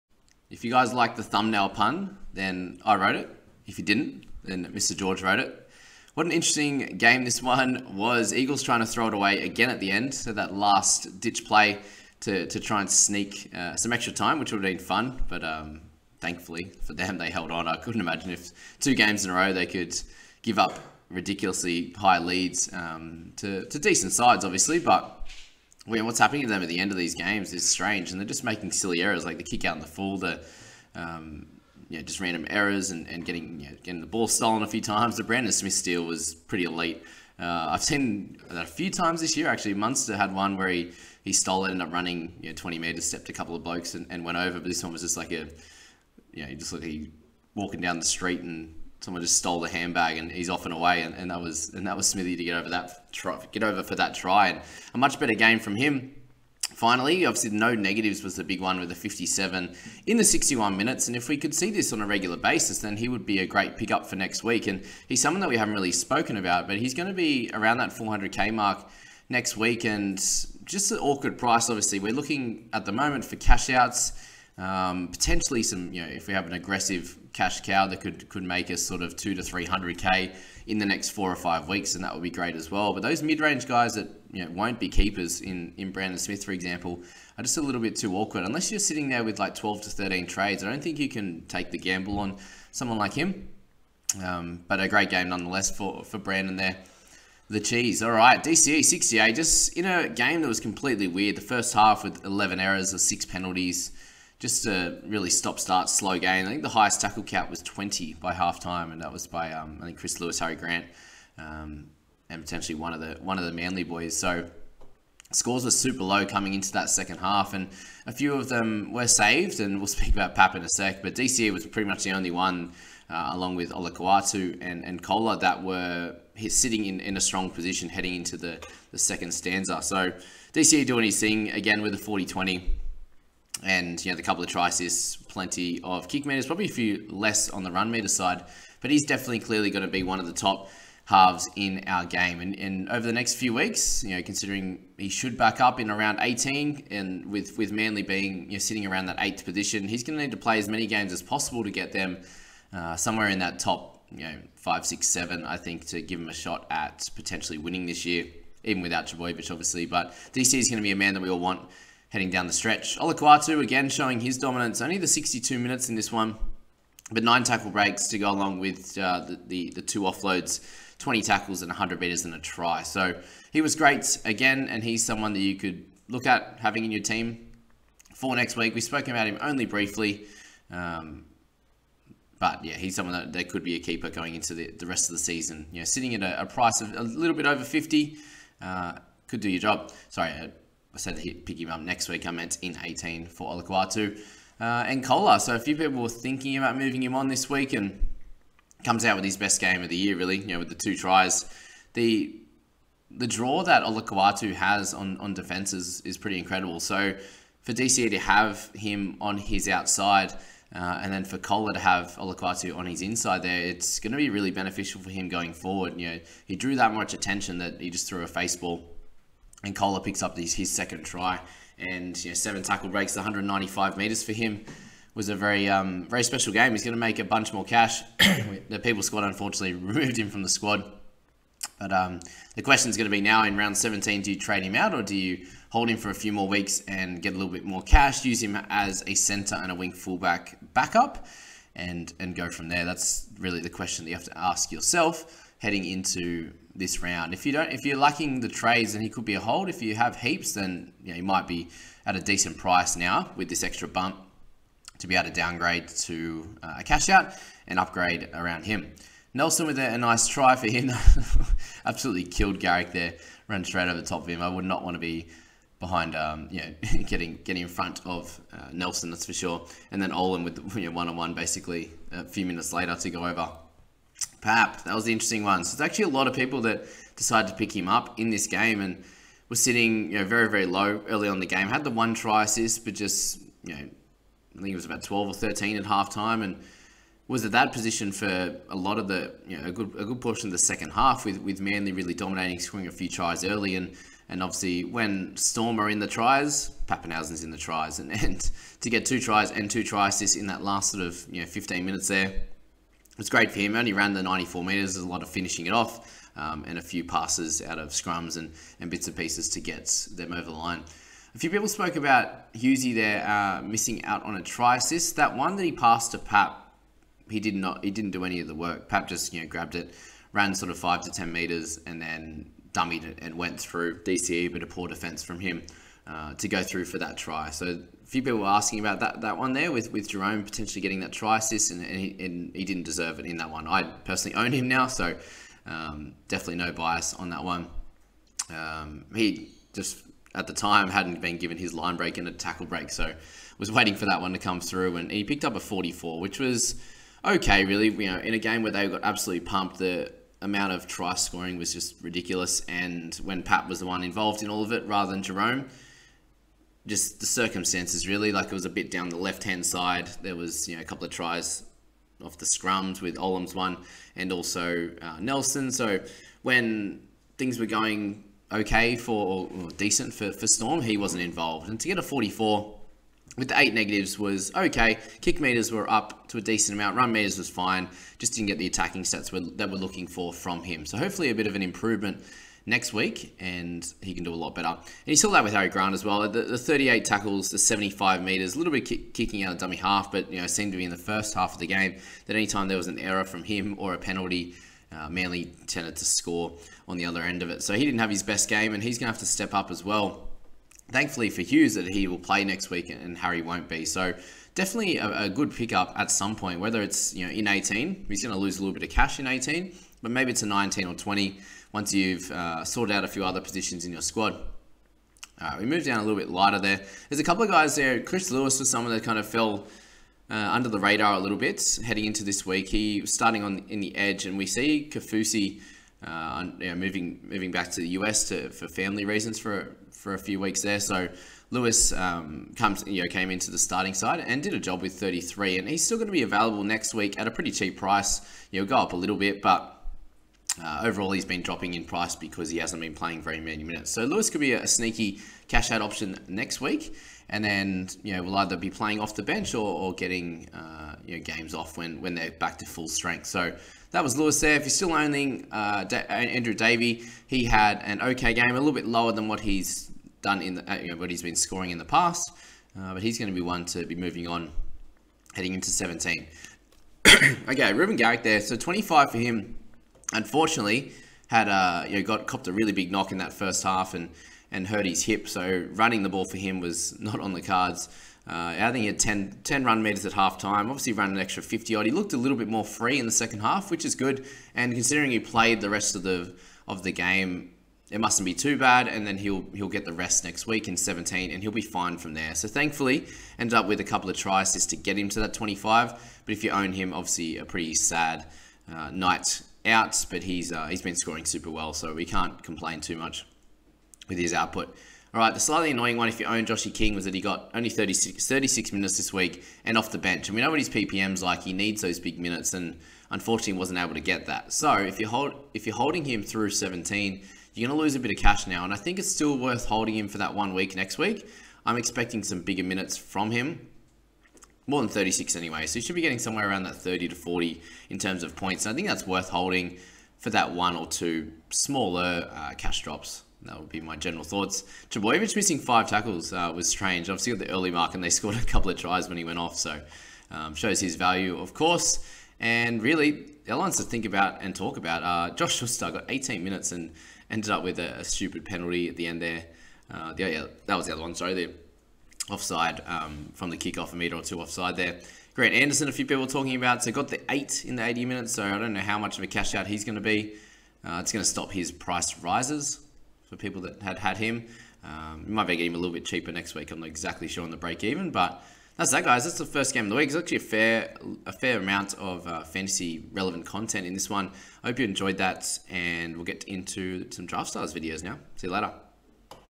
When is the best time to pick up DCE? Thursday Night Wrap!
I go through the first game of the round :)